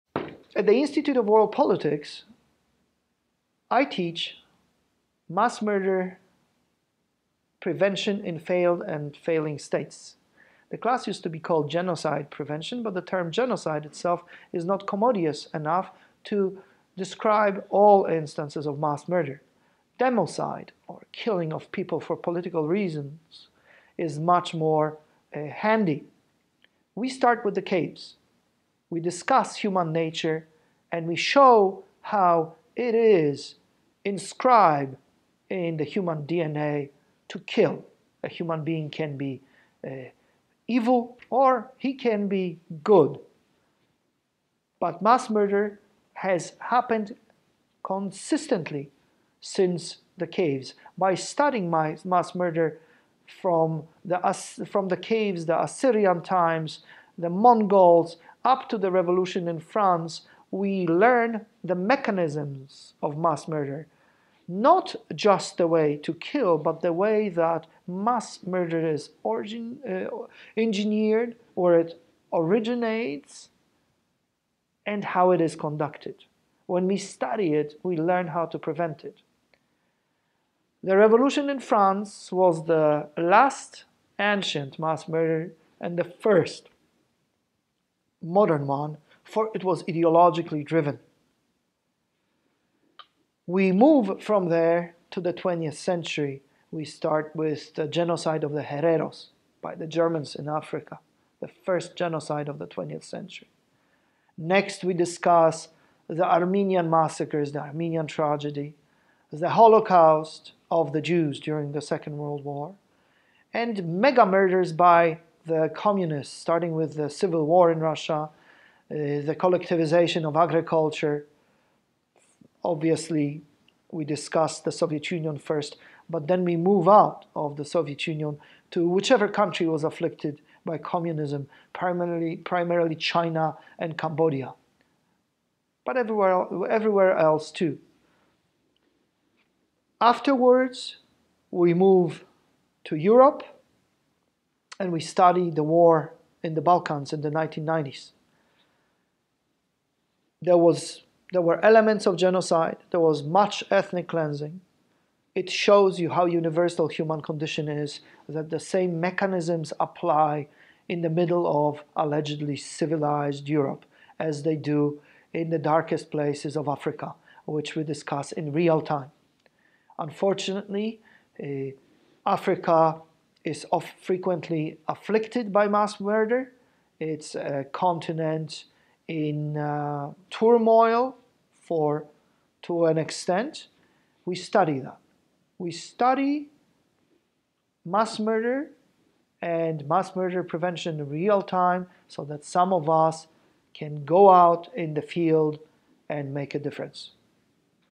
Mass Murder Prevention in Failed and Failing States (IWP 649)
Marek Jan Chodakiewicz discusses his course on Mass Murder Prevention in Failed and Failing States (IWP 649).

This seminar concentrates on genocide and genocide prevention in the 20th and 21st centuries. We shall commence by examining legal and cultural definitions of nations and of genocide (Rafal Lemkin’s theory). Next, we shall briefly examine genocide in history, starting with the Biblical Times, including the Assyrians, and ending with the Revolution in France, the Vandean auto-genocide in particular. Then, we shall describe various instances of genocide, including the Armenian massacres, the Ukrainian famine, the Jewish Holocaust, the Soviet and Nazi “ordinary terror” against Catholic Polish elite and people, grass-roots ethnic cleansing campaigns in the Ukraine during the Second World War, state-mandated ethnic cleansing of the German population in the wake of World War II, ethnic cleansing in the Balkans in the 1940s and 1990s, self-genocide in China and Cambodia, as well as the massacres in Rwanda and Darfur. In the context of extermination, we shall also discuss mass deportations, expropriations, and cultural genocide.

The objective is to conduct case studies of genocide, identify ideological and political reasons for those crimes, and detect early warning signs for genocide prevention.